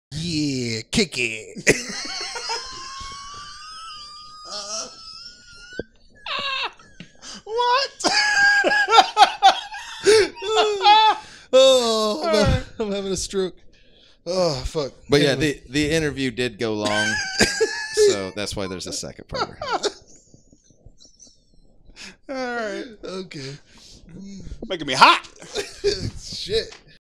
Kick It!
Frank tries a new bold transition into "The Reading Rainbow Of Death" segment (Formerly "Jared's Realm")and Jared gets visibly irritated when Jason and Frank both crack up and ruin his introduction to the interview with Jonathan Raab.